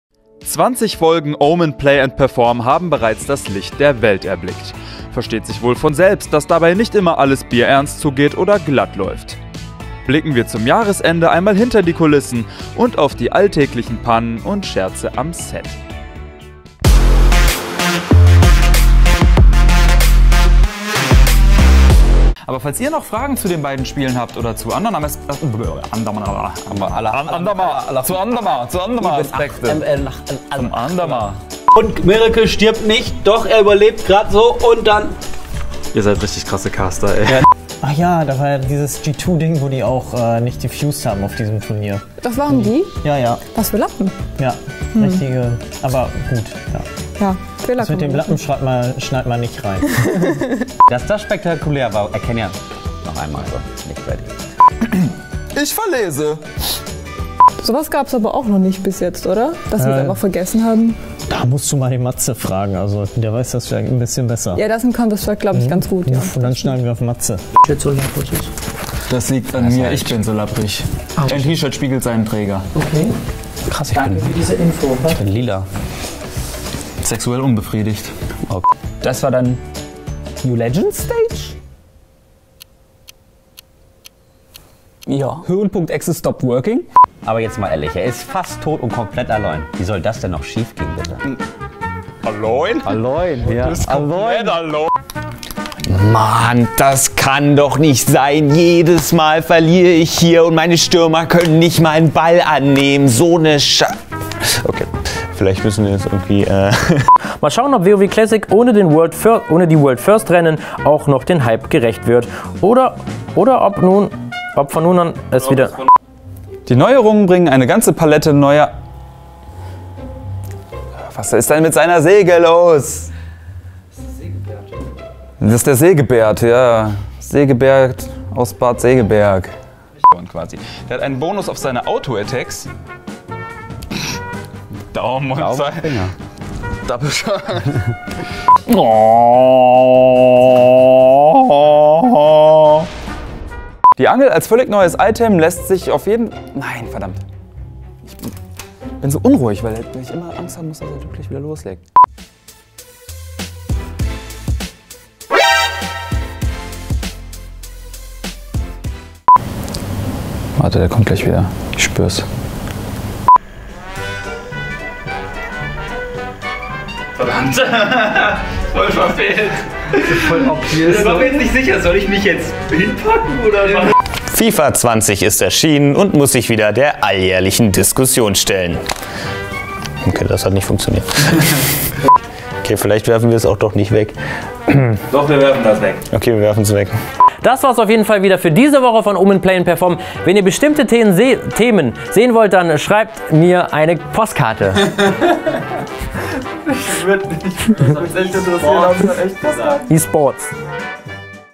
"Was für Lappen ey" - Outtakes 2019 - OMEN Play + Perform | esports.com
Esports und perfekte Abläufe? Das passt natürlich selten zusammen. Bei OMEN Play + Perform sind wir zwar nah an der Perfektion, dennoch klappt nicht immer alles. Die besten Aussetzer 2019 haben wir für euch zusammengeschnitten